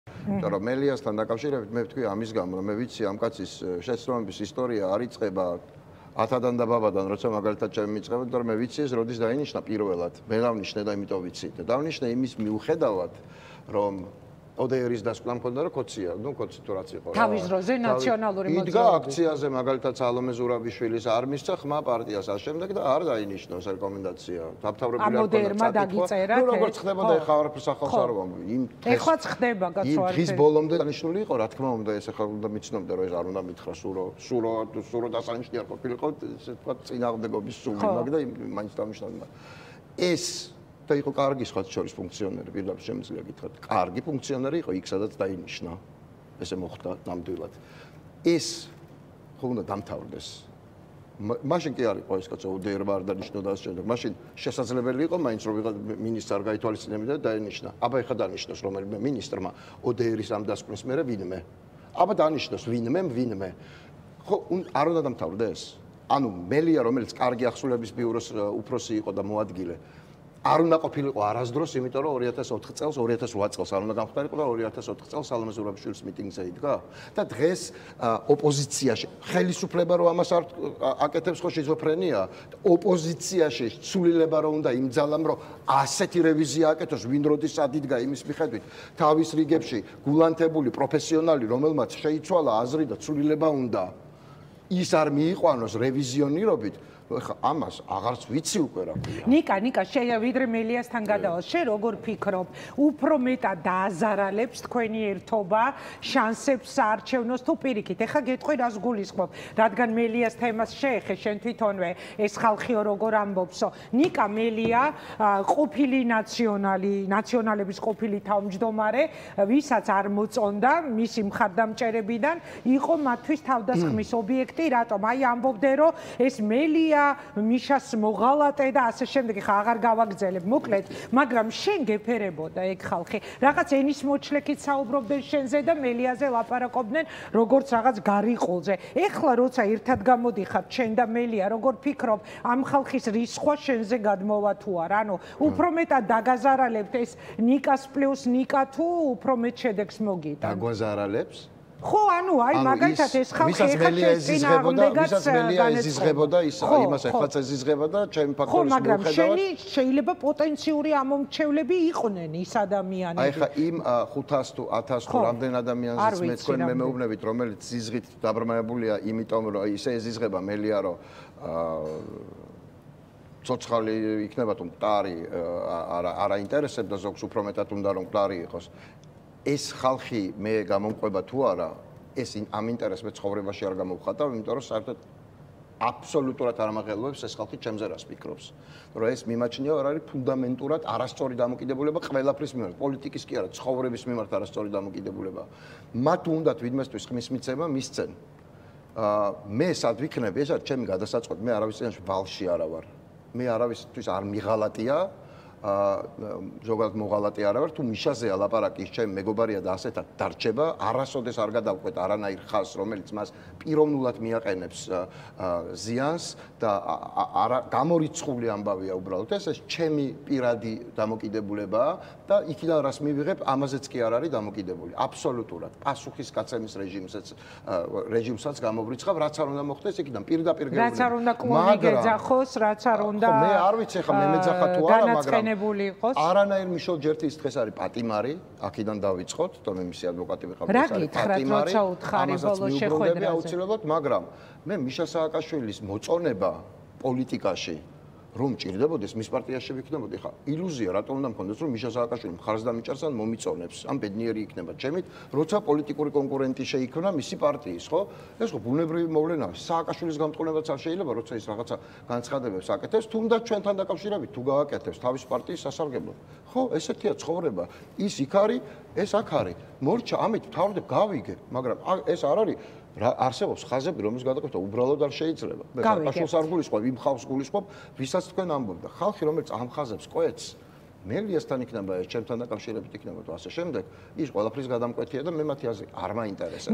ნიკა გვარამია - სააკაშვილის მოწონება რომ მჭირდებოდეს პოლიტიკაში, მის პარტიაში ვიქნებოდი
სააკაშვილის მოწონება რომ მჭირდებოდეს პოლიტიკაში, მის პარტიაში ვიქნებოდი - ნიკა გვარამია